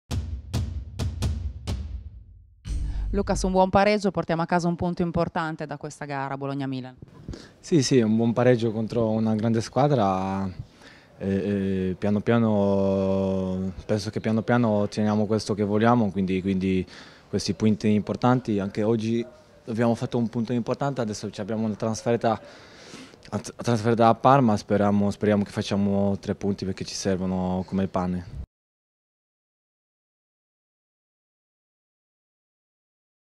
BFCMilan: le dichiarazioni di Skorupski
L'intervista al portiere rossoblù al termine della gara.